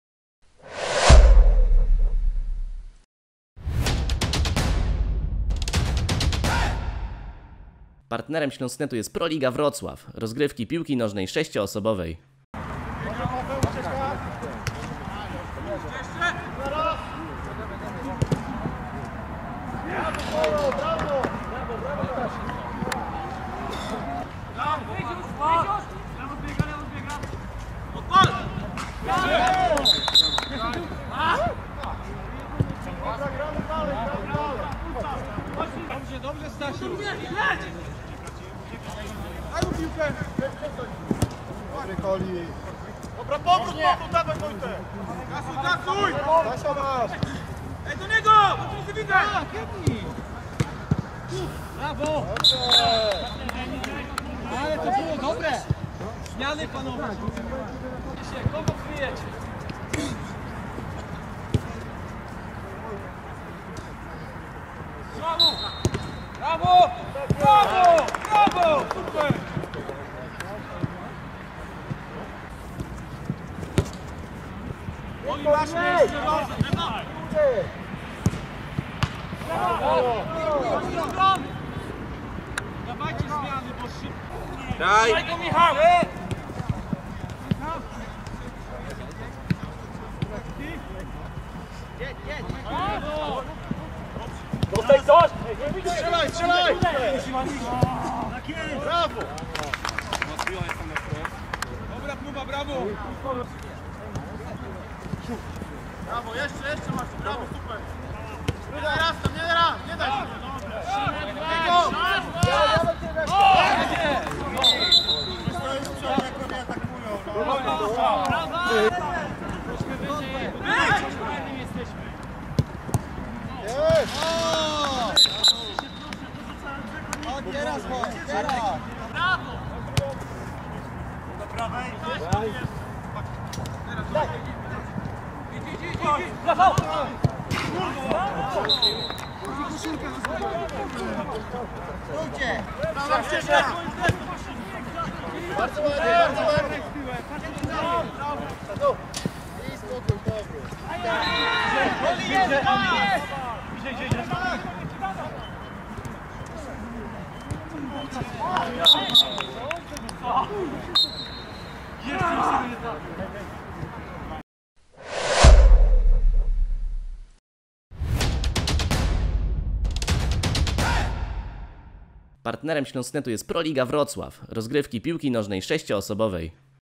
Skrót meczu Śląsknet 0:5 Elektro-Bud (PRO Liga Wrocław)
W przedostatnim meczu grupy spadkowej w PRO Lidze ponownie borykaliśmy się z dużymi problemami kadrowymi. Efekt? Niestety przegraliśmy z Elektro-Bud 0:5. Został przed nami ostatni mecz, w którym zmierzymy się z Force Team. Miejmy nadzieje że zakończymy sezon pozytywnie i może strzelimy jakiegoś gola.

✌️ DOCENIASZ NASZĄ PRACĘ? ZOSTAŃ PATRONEM!

💰 CHCESZ ZOSTAWIĆ JEDNORAZOWY NAPIWEK?

🎥Montaż: Dominik Poprawski